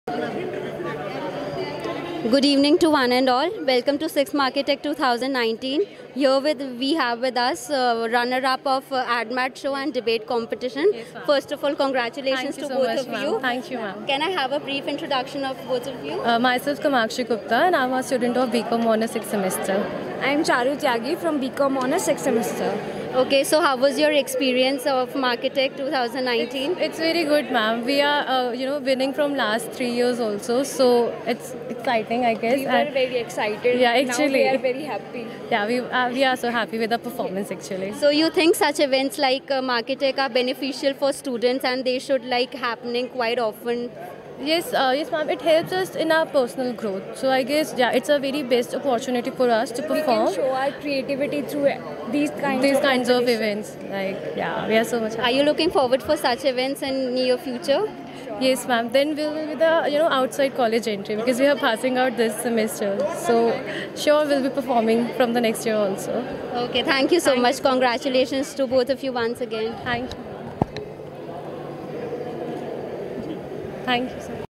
MARKETECH-2019 | 6th Management IT Inter College Fest | CPJ COLLEGE | IP UNIVERSITY
Ms Sonali Choudhary, Assistant Professor –CPJ School of law interacting with 𝐑𝐮𝐧𝐧𝐞𝐫-𝐮𝐩 𝐨𝐟 𝐌𝐚𝐝 𝐀𝐝𝐯𝐞𝐫𝐭𝐢𝐬𝐢𝐧𝐠 𝐂𝐨𝐦𝐩𝐞𝐭𝐢𝐭𝐢𝐨𝐧 @MARKETECH - 2019 : the 6th Management IT Inter College Fest organized by Chanderprabhu Jain College of Higher Studies & School of Law on April, 5th- 6th 2019.